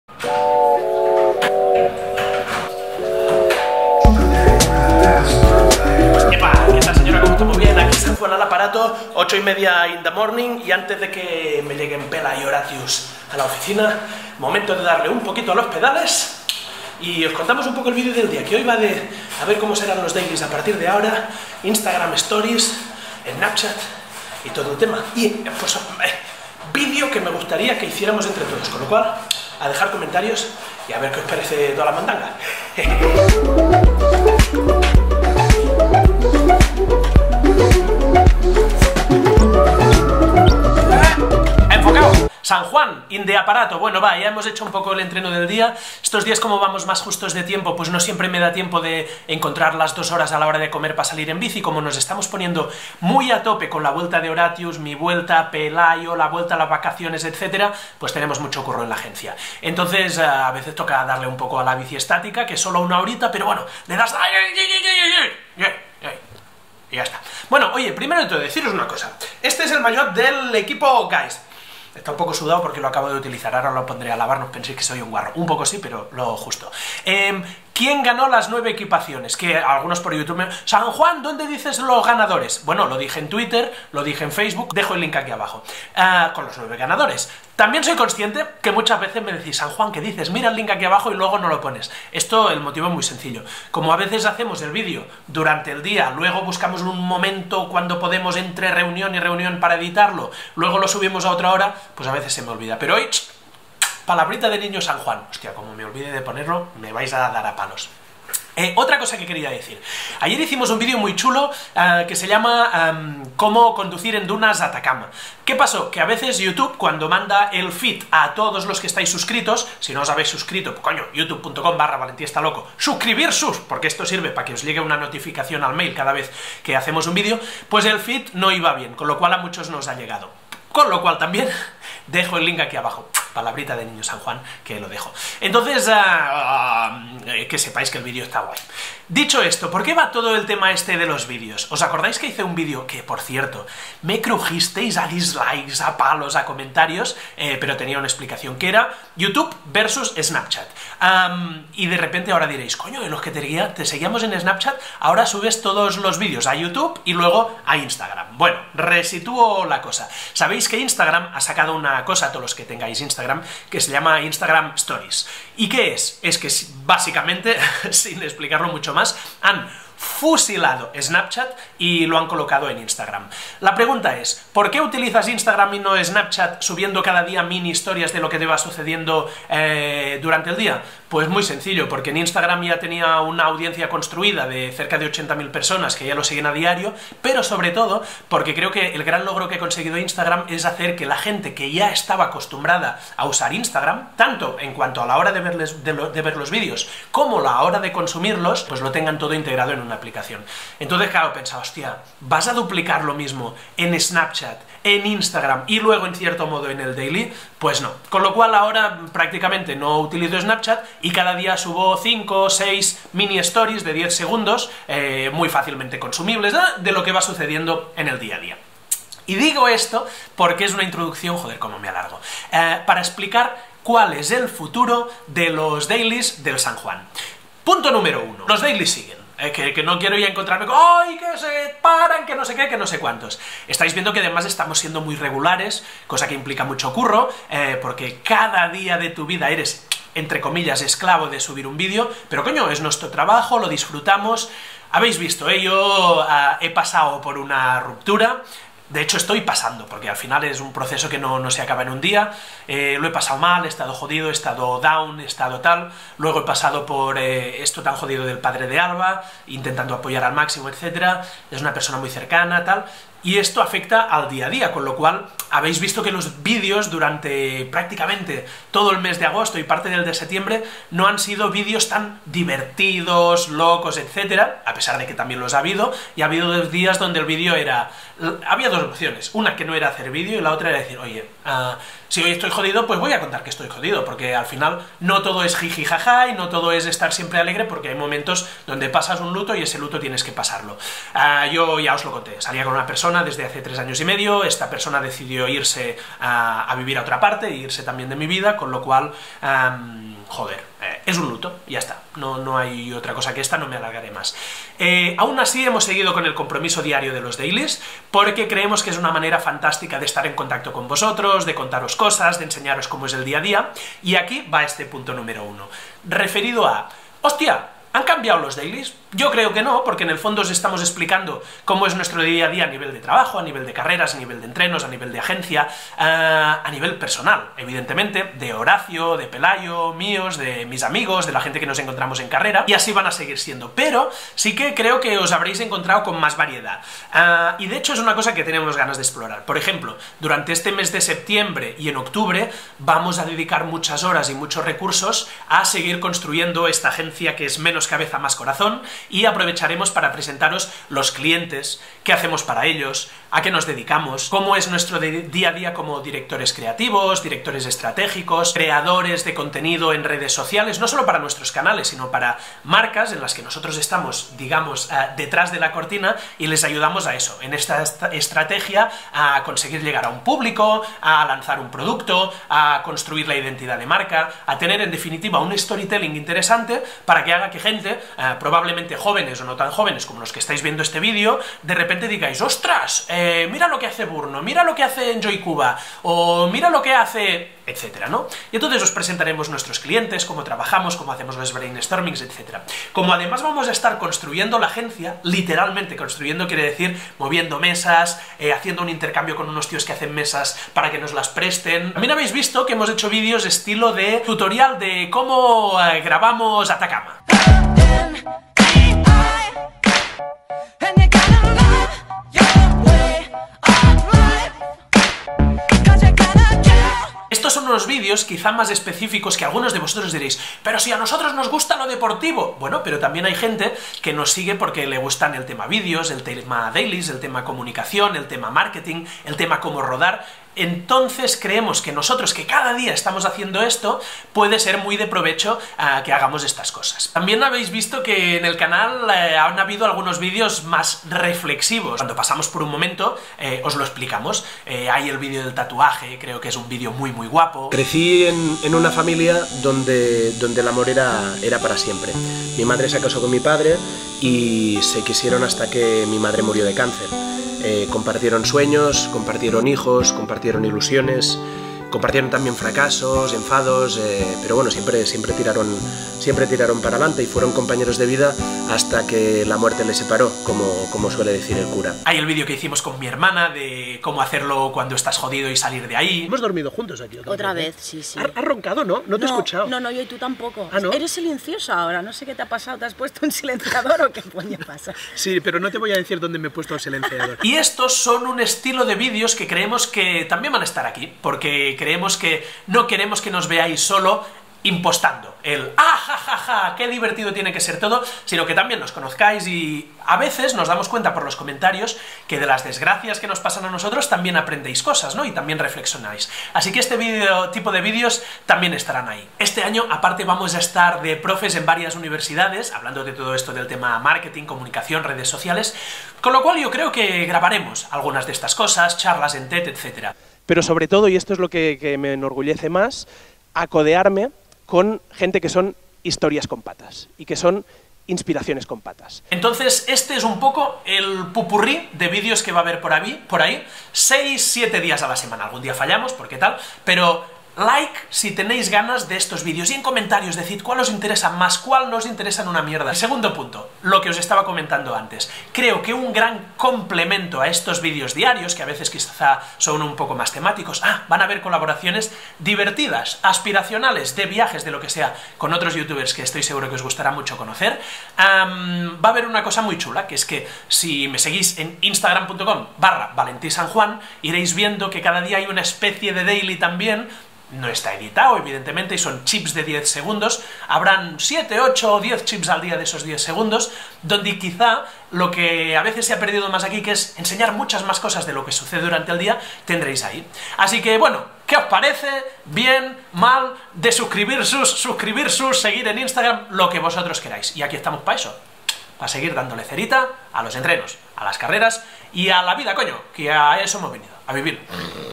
El Futuro De Mis Vídeos Diarios
Para todos los que preguntáis qué va a pasar con los vídeos diarios que subimos a youtube, y por qué cada vez le damos más caña a Instagram Stories, aquí va un vídeo en el que me gustaría que colaborarais :))